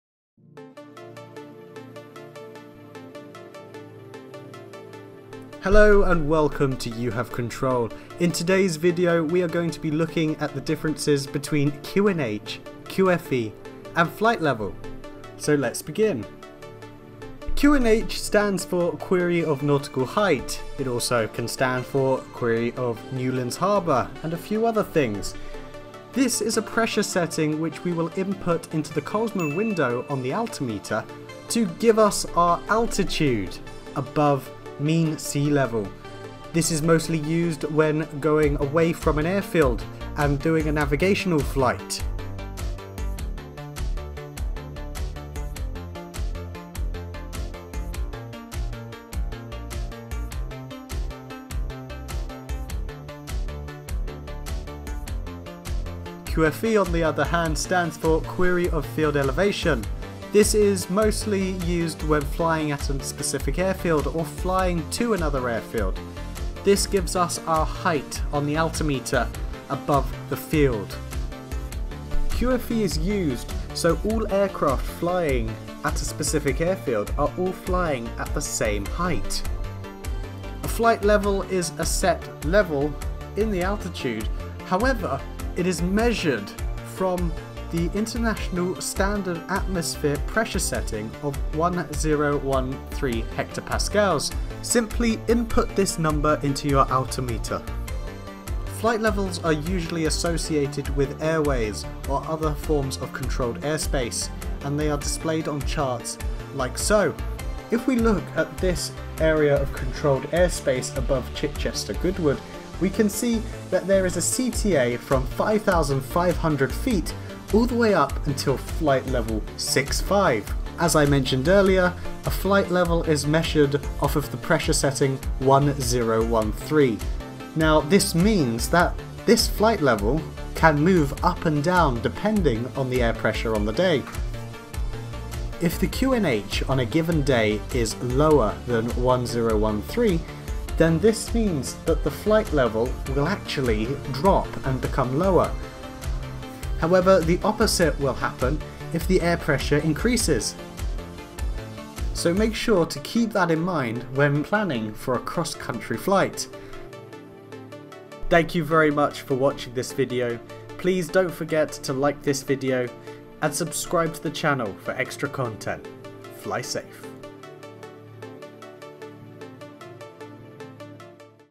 QNH?? QFE?? What are they?
In this video we talk in detail about the difference between QFE and QNH. We also will look into flight levels.